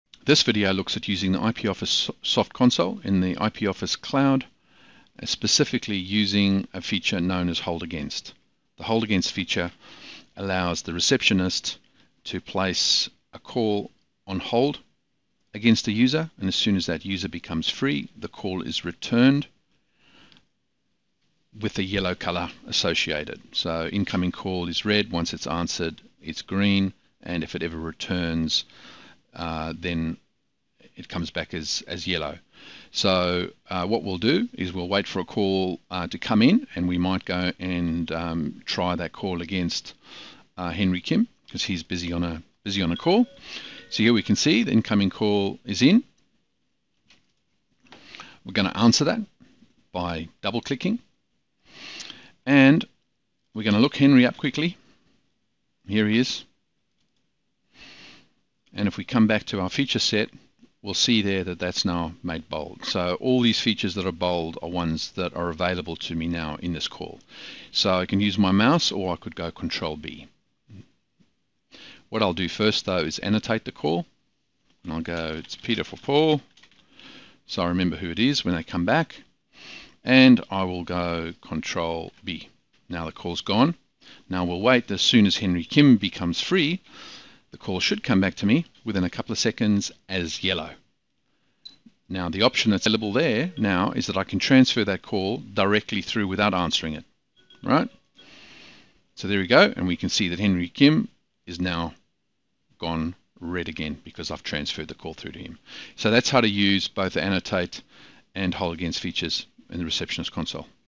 Avaya IP Office SoftConsole
This video looks at how to use the hold against feature in the IP Office Soft Console connected to the Avaya Cloud.  Hold against provides an operator the flexibility to place a call on hold against an extension that is busy.  Using this feature, as soon as the extension becomes free,  the call is returned to the operator and highlighted in yellow(so as to be distinguishable from a new incoming call).  The operator then has the choice to single click transfer to this user or answer the call and supervised transfer, conference etc.